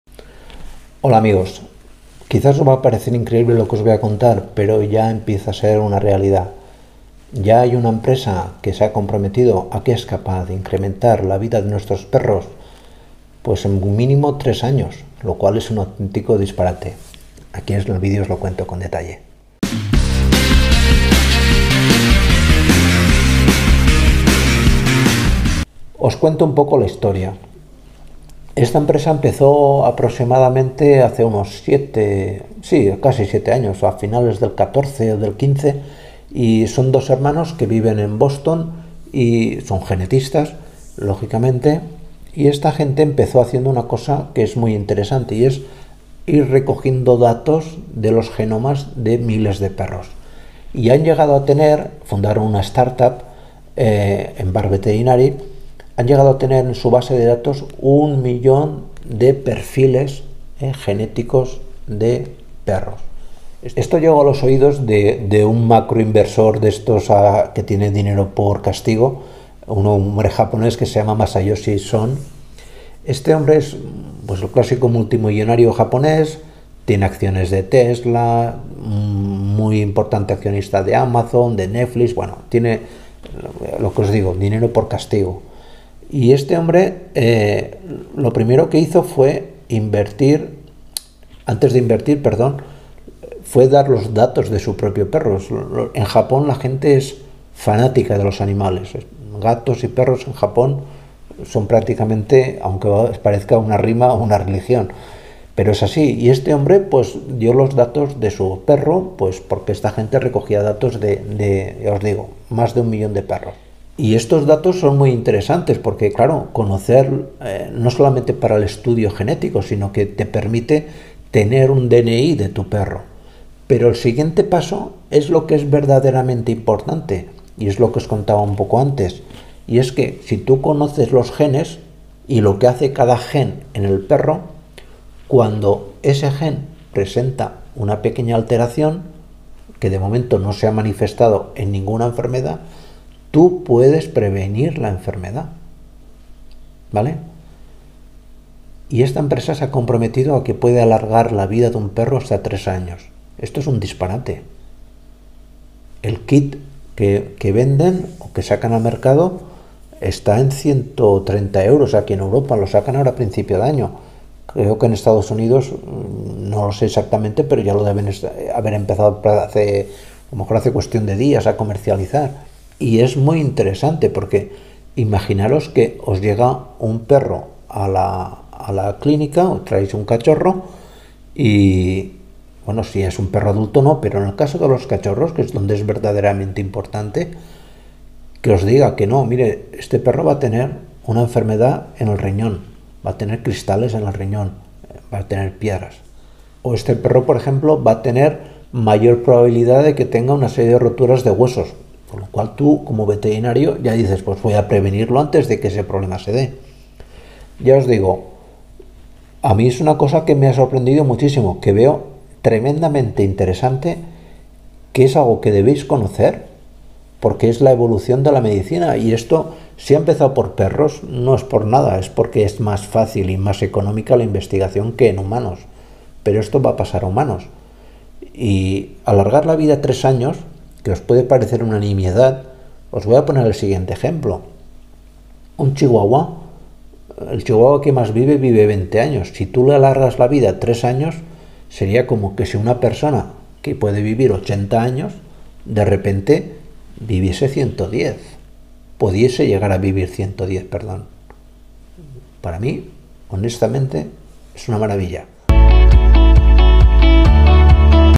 ❤️ ESTA EMPRESA CONSIGUE QUE TU PERRO PUEDA VIVIR MÁS AÑOS ❤️
Aquí te explico ➡️.➡️ como una ❌Empresa es Capaz de hacer que un Perro Viva Más ❌

00:00 ESTA EMPRESA PUEDE ALARGAR LA ESPERANZA DE VIDA DE TU PERRO
00:29 LA HISTORIA DE EMBARK VETERINARY
01:06 MASAYOSHI SON INVIERTE EN EMBARK VETERINARY
01:56 LA IMPORTANCIA DE LOS GENES EN LAS ENFERMEDADES DE LOS PERROS
02:56 LA IMPORTANCIA DE LA PREVENCIÓN PARA ALARGAR LA VIDA DE UN PERRO